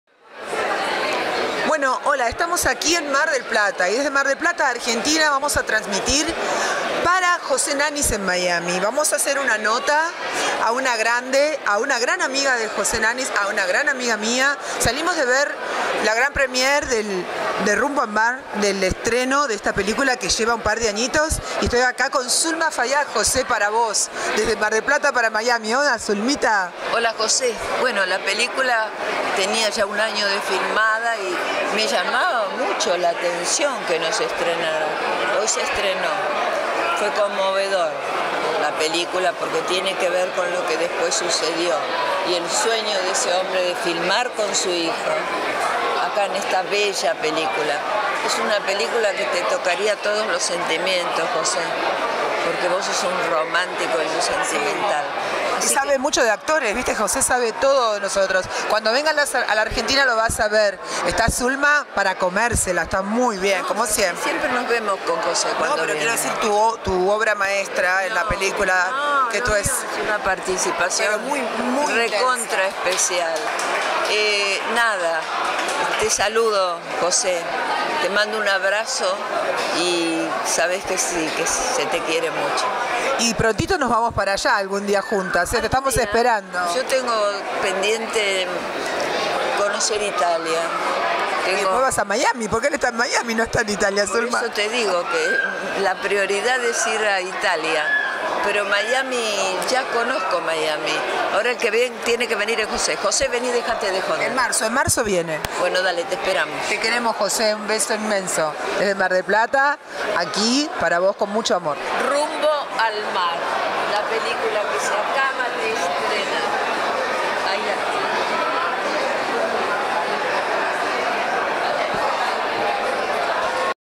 SANDRA DOMINGUEZ CON ZULMA FAIAD - EXCLUSIVA NOTA A MIAMI
DESDE MAR DEL PLATA SANDRA DOMINGUEZ EN UNA NOTA EXCLUSIVA A ZULMA FAIAD PARA BUENISIMO TV MIAMI